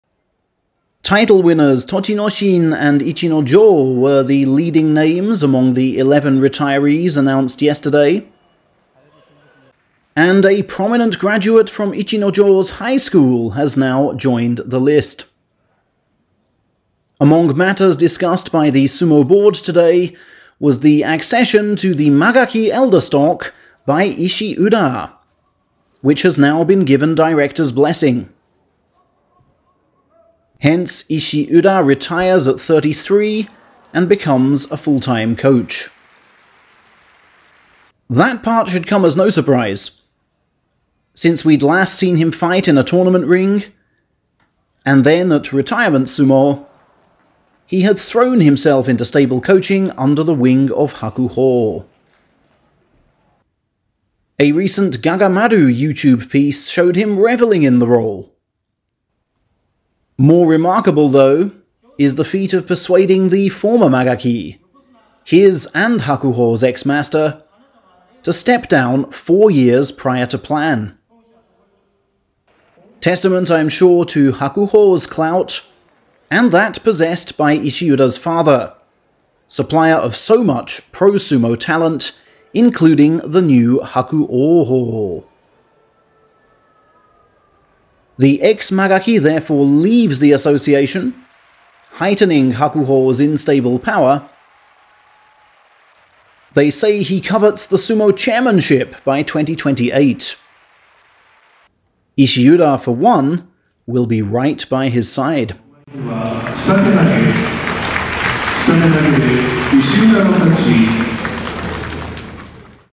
Muscular Ishiura retires at 33
The pocket-sized muscle man had not been seen in the ring for 14 months, and was presumably exploring various ways to secure his current coaching position. Job very well done!